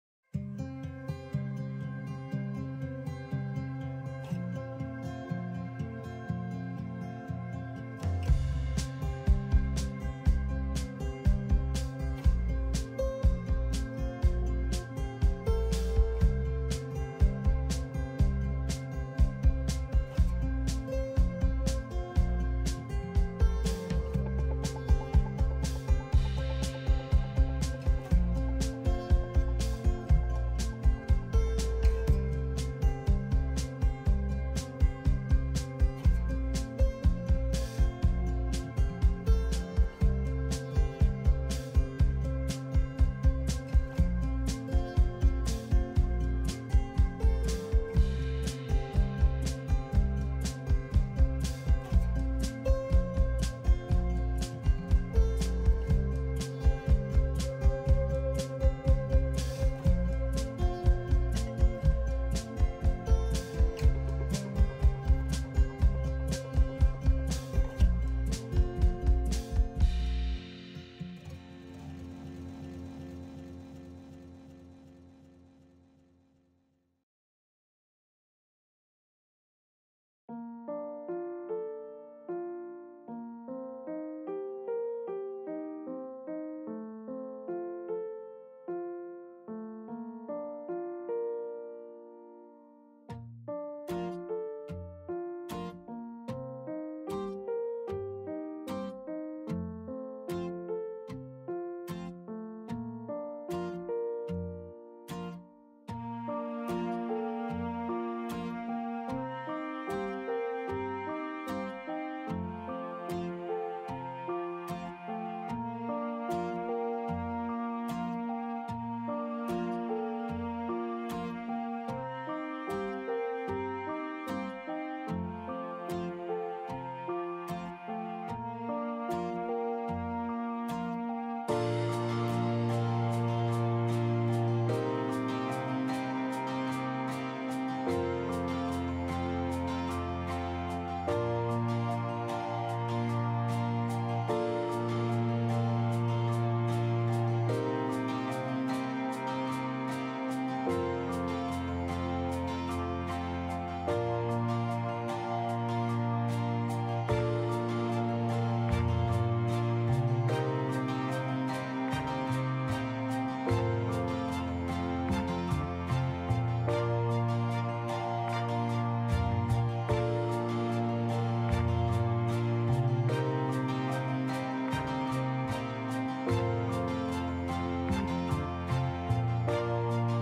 [ 3 minute Watercolor ] Landscape Watercolor - Lighthouse ( color name view ) NAMIL ART
[ 3분 수채화 ] 풍경 수채화 - 등대가 있는 바다 (스케치 & 칼라 이름 보기, 수채화재료 소개)남일수채화